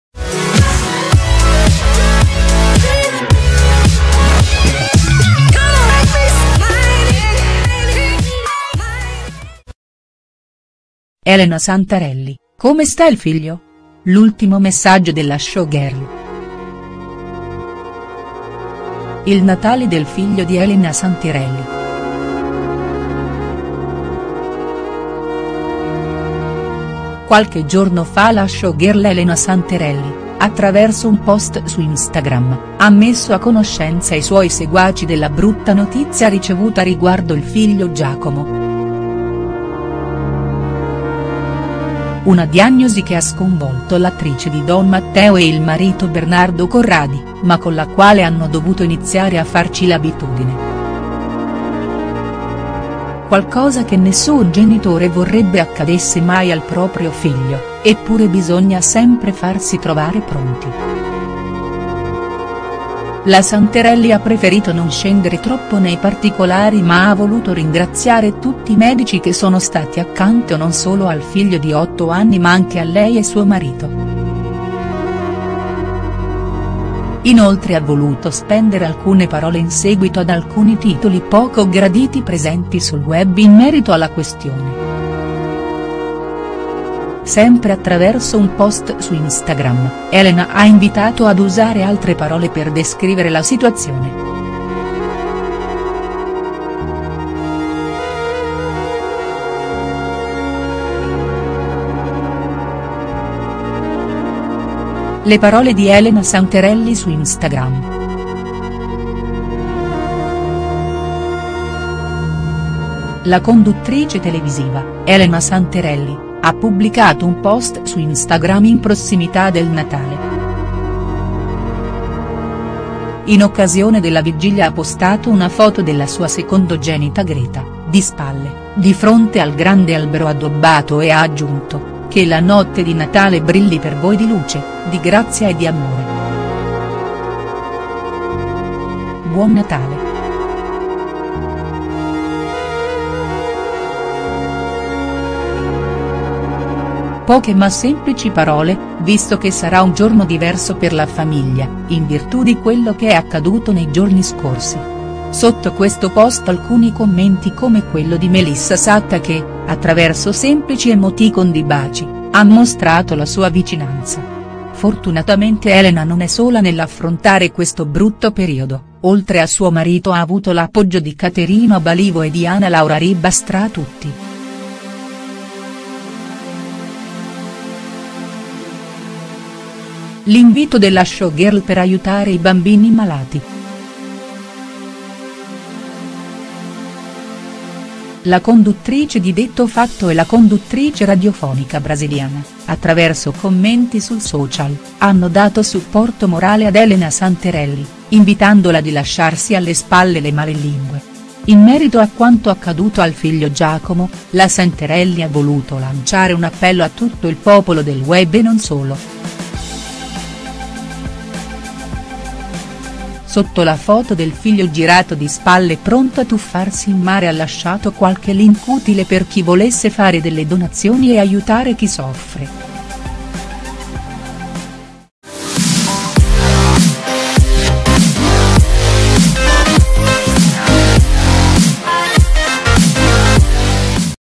Elena Santarelli, come sta il figlio? L’ultimo messaggio della showgirl| STARS NEWS
Elena Santerelli fa gli auguri di Natale su Instagram e pensa al figlio. Poche parole ma concise.